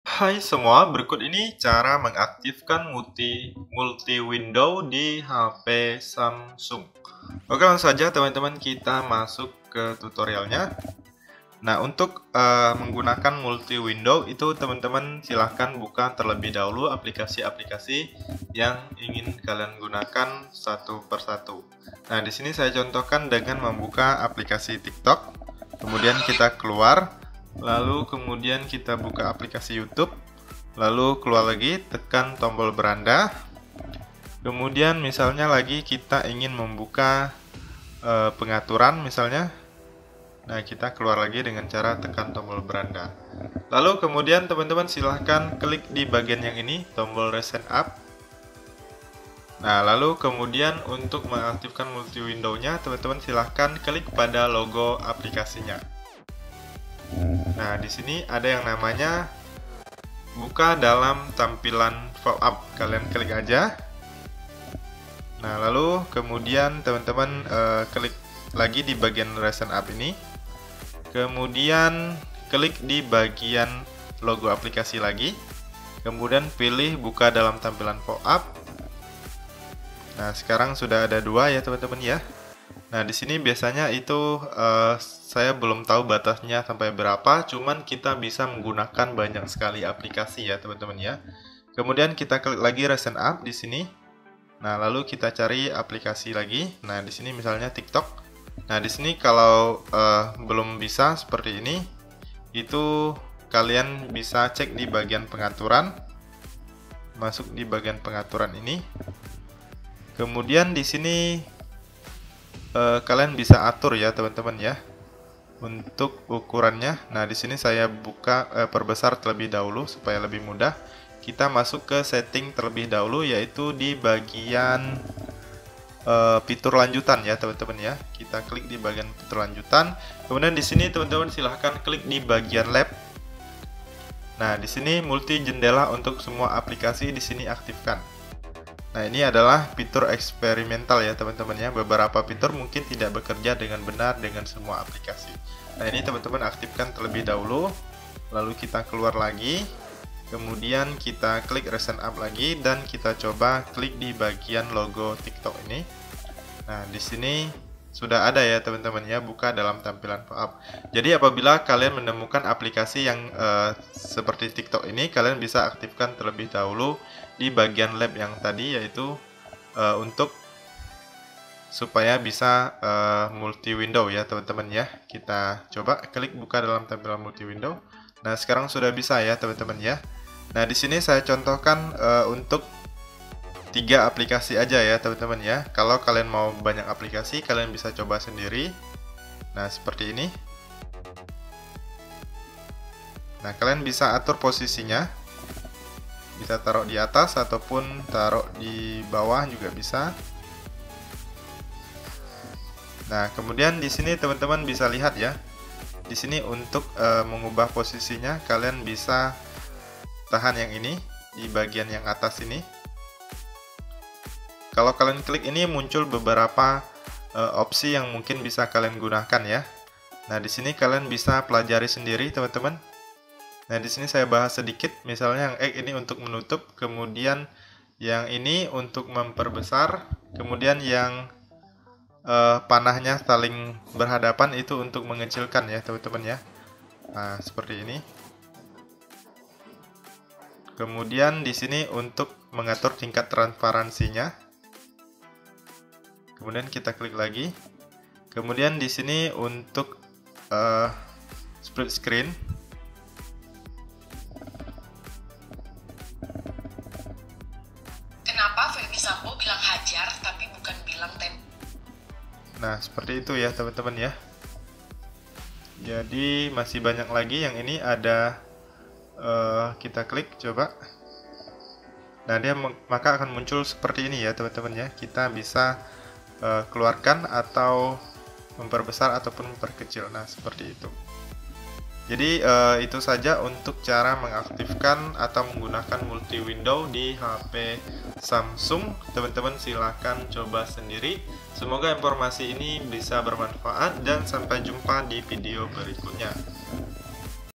Cara Multi Window di Hp Samsung A33 5G
Berikut ini cara mengaktifkan multi window di hp Samsung A33 5G.
semoga bermanfaat 🙏.